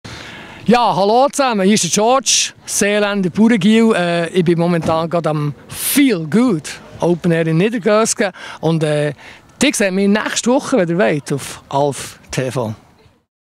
Trailer Alf-TV mit George
Trailer für den Konzertmitschnitt von George im Arolfinger Lokalfernsehen Alf-TV.